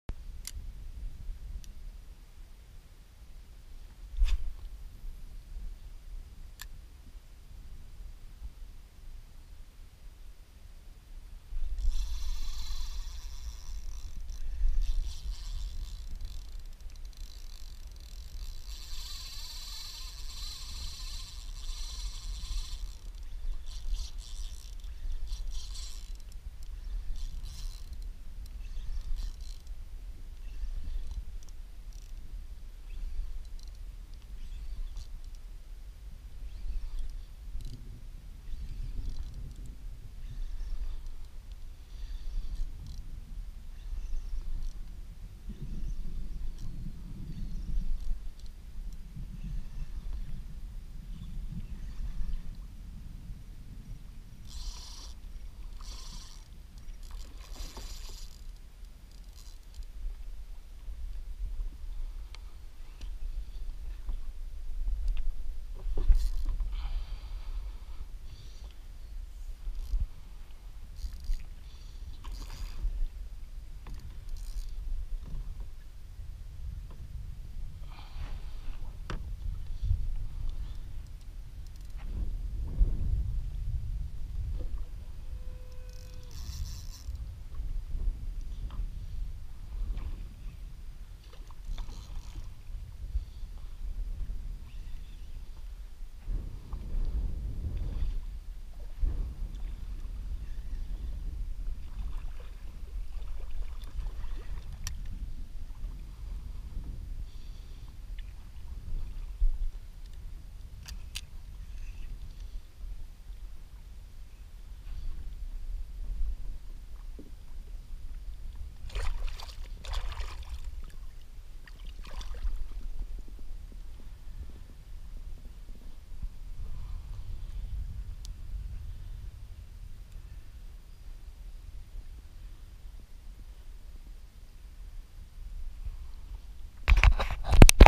Mosquito Lagoon solo trip - Redfish 7/18/17
Reds still eat in thunderstorms. Using a doa cal